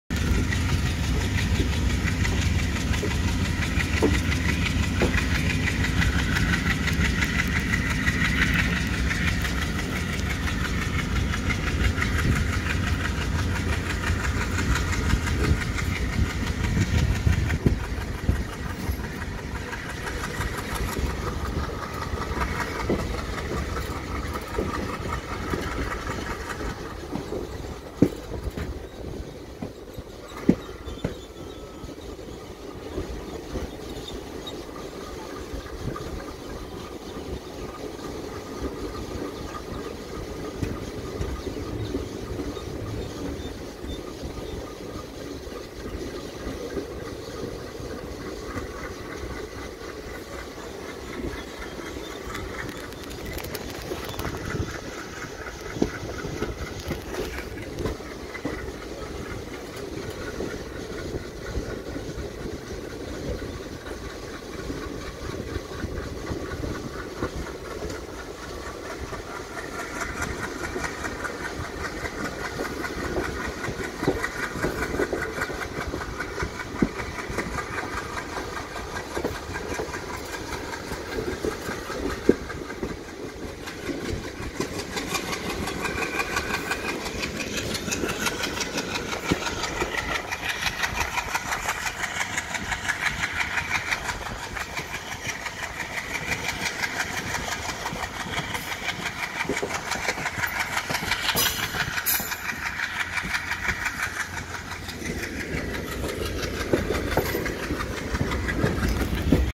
Train leaving Pollachi Junction, Tamil Nadu | Southern Railway | Travel with Jayant
A short video of Amritha Express leaving Pollachi Junction in the awesome weather in Tamil Nadu.